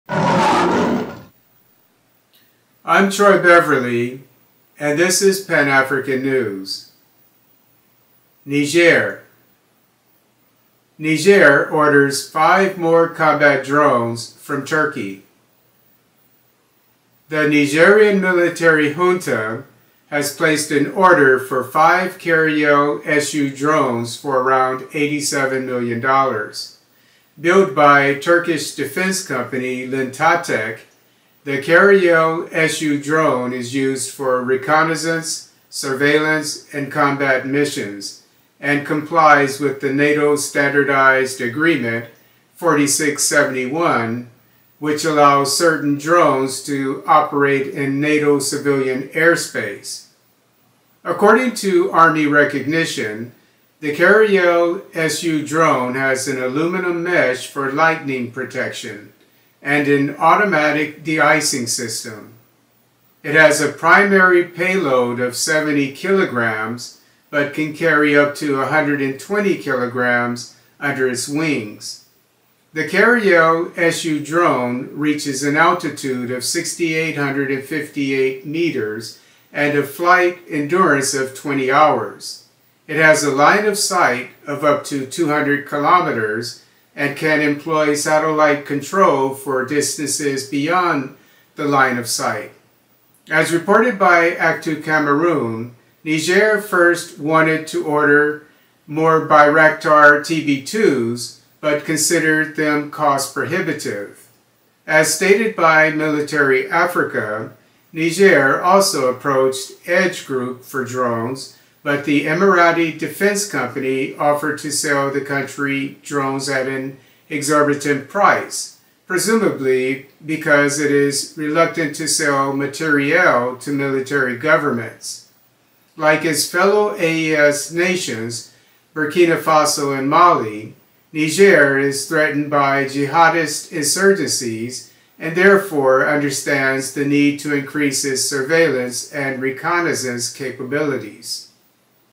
Niger orders 5 more combat drones from Turkey
The Nigerien military junta has placed an order for five Karayel-SU drones for around $87 million. Built by Turkish defense company Lentatek, the Karayel-SU drone is used for reconnaissance, surveillance, and combat missions and complies with the NATO Standardized Agreement-4671, which allows certain drones to operate in NATO civilian airspace. According to Army Recognition, the Karayel-SU drone has an aluminum mesh for lightning protection and an automatic deicing system. It has a primary payload of 70 kilograms but can carry up to 120 kilograms under its wings. The Karayel-SU drone reaches an altitude of 6,858 meters and a flight endurance of 20 hours. It has a line of sight up to 200 kilometers and can employ satellite control for distances beyond the line of sight. As reported by Actu Cameroun, Niger first wanted to order more Bayraktar TB2s but considered cost prohibitive. As stated by Military Africa, Niger also approached EDGE Group for drones, but the Emirati defense company offered to sell the country drones at an exorbitant price, presumably because it is reluctant to sell matériel to military governments. Like its fellow AES nations Burkina Faso and Mali, Niger is threatened by jihadist insurgencies and therefore understands the need to increase its surveillance and reconnaissance capabilities.
Sources:
Niger Orders Several Drones from Türkiye Amid Tensions in Region
Army Recognition
Niger reportedly orders 5 Turkish drones in $87M deal
Turkiye Today
Nigér to acquire Turkish Karayel combat drones
Military Africa
Niger to acquire several Turkish-made drones to modernize its army
Actu Cameroun